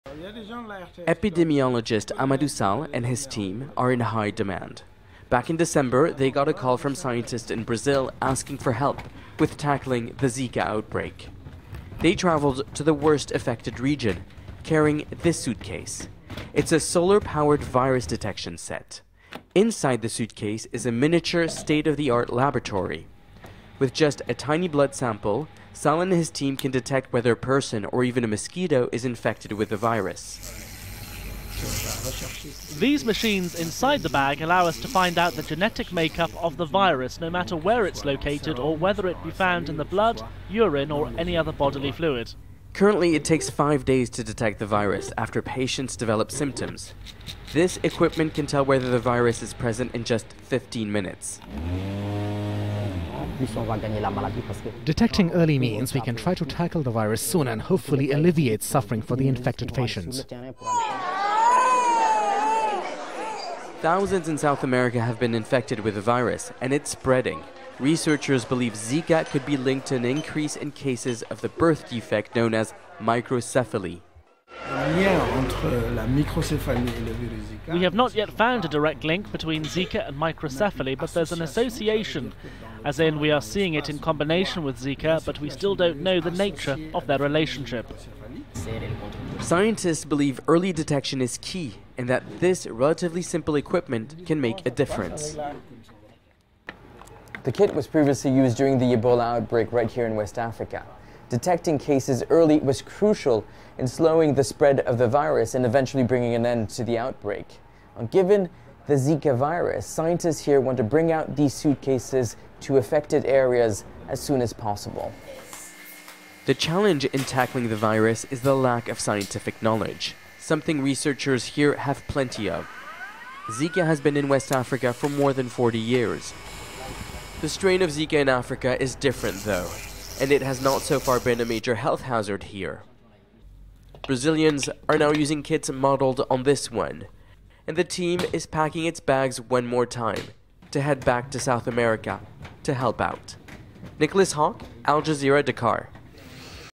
Senegal pioneers Zika virus detection system
Scientists in West Africa are hoping to find ways to stop the spread of the Zika virus.

Researchers in Senegal are helping people in Latin America affected by the virus, by using similar technology used to detect Ebola.

Nicolas Haque reports from Dakar.